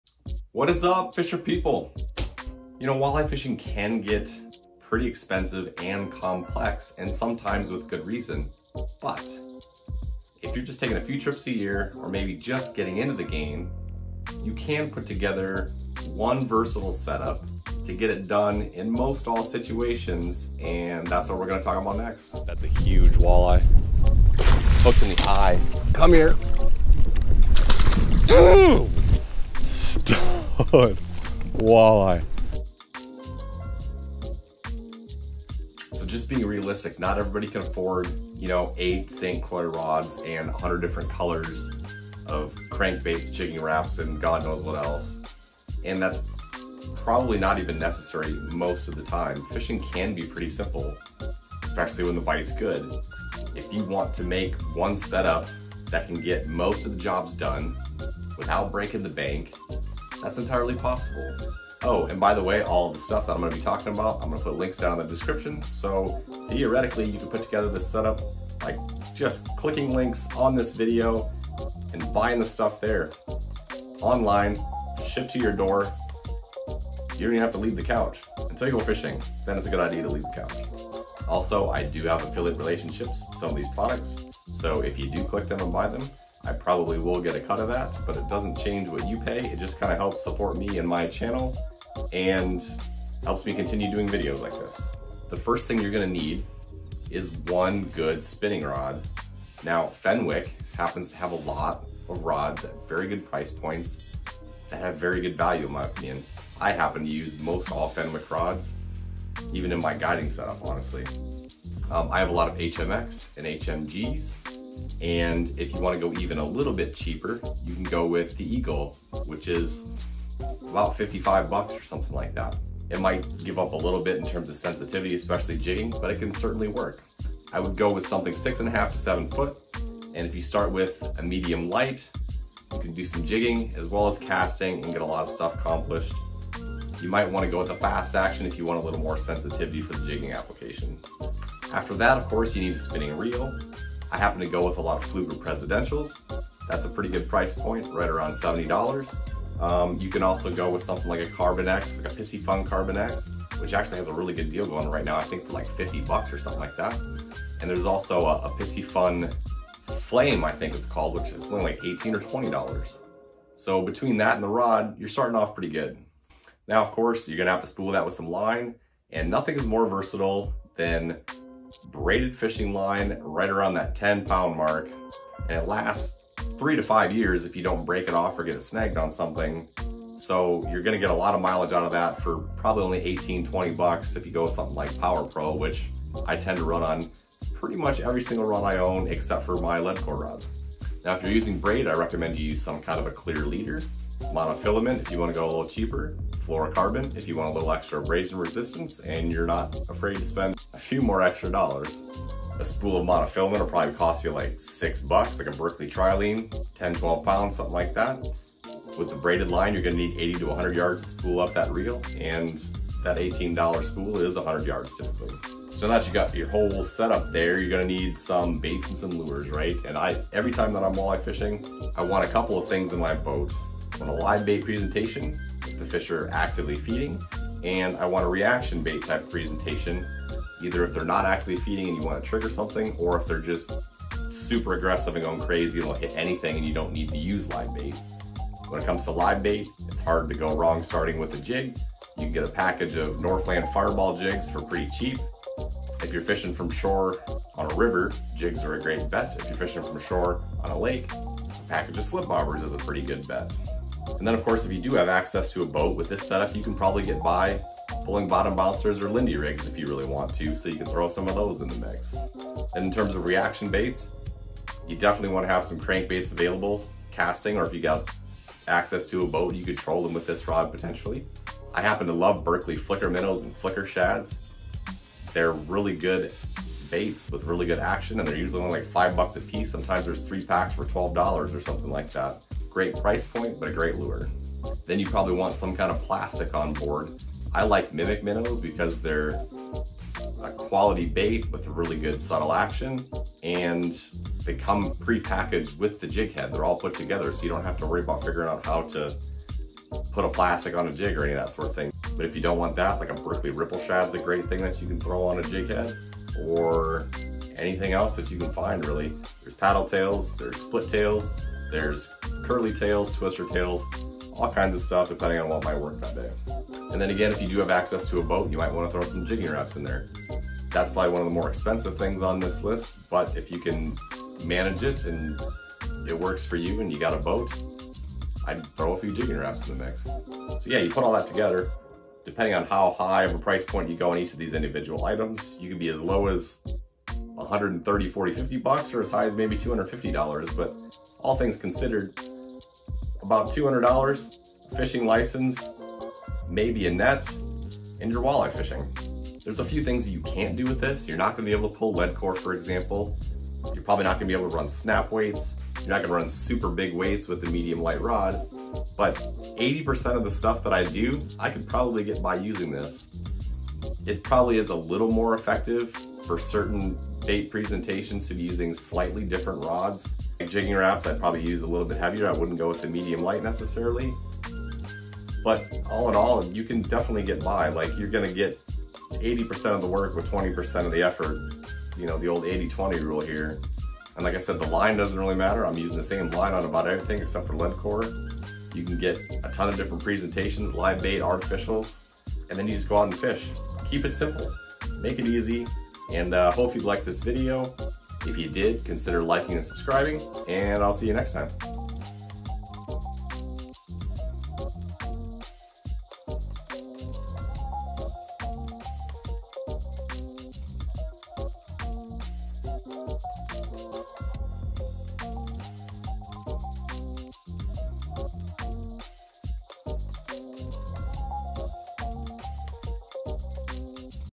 One All-around Walleye Setup on a Budget
Whether you're just getting into the game or only have space to carry around one good rod/reel combo and small tackle box, I'll show you how to put together a package that can catch walleyes in most situations, whether from shore or in a boat. Could be a great Christmas list for a new or occasional fisher person too!

Fenwick Eagle Spinning Rod

Fenwick HMG Spinning Rod

Pfleuger President Spinning Reel

Piscifun Carbon X Spinning Reel

Piscifun Flame Spinning Reel

Power Pro Braided Fishing Line

Berkley Trilene XT Mono Fishing Line

Northland Fireball Jig Kit

VMC Hammerhead Jigs

Thill Slip Bobber Kit

Spinner Rig

Lindy Rig

Berkley Flicker Minnow

Rapala Jigging Rap

Mimic Minnow